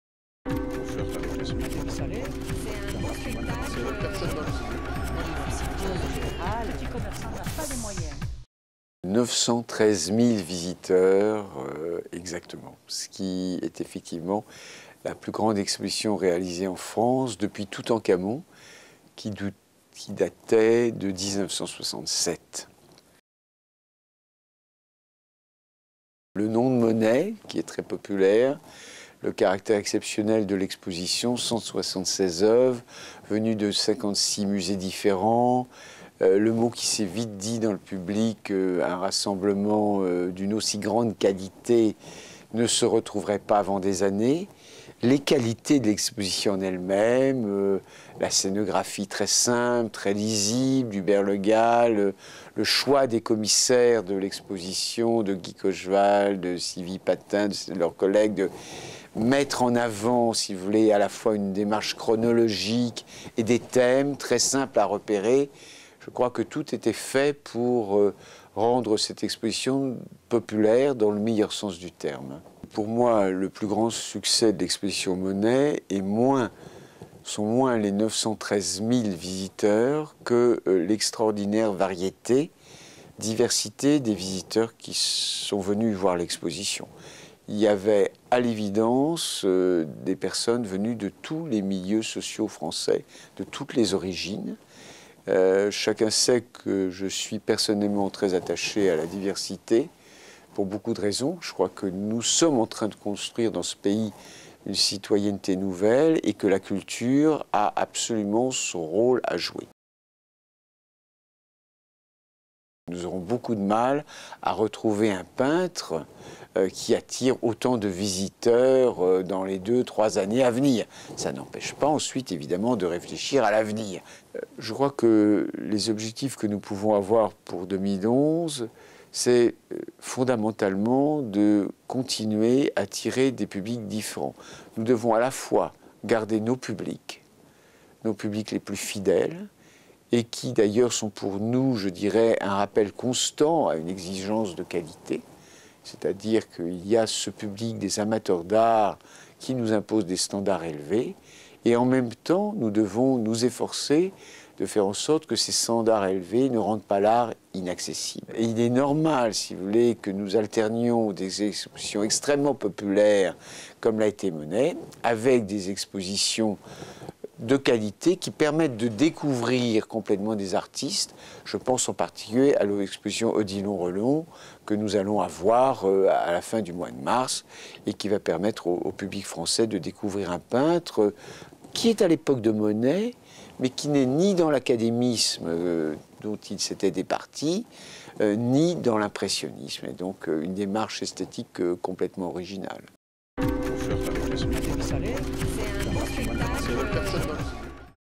Monet au Grand Palais : l'exposition record - Le Figaro
Vidéo - Avec 913.064 visiteurs en quatre mois, l'impressionniste talonne Toutankhamon et ses 1,2 million de visiteurs en 1967 (l'exposition avait alors duré six mois). Jean-Paul Cluzel, récemment nommé Président de la Réunion des musées nationaux et du Grand Palais, revient sur ce succès et présente ses objectifs pour l'année 2011.